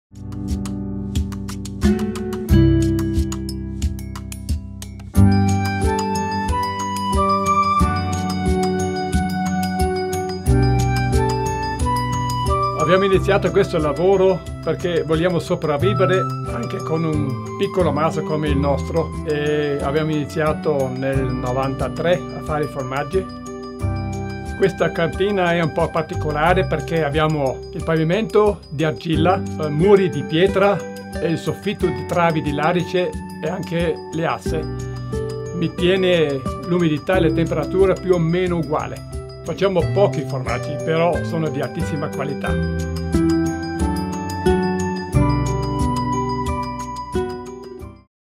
My favorite farmer
Heinrich Schneider of the Auener Hof in Sarentino shows one of the places where he takes local products for his special cuisine at the dining home.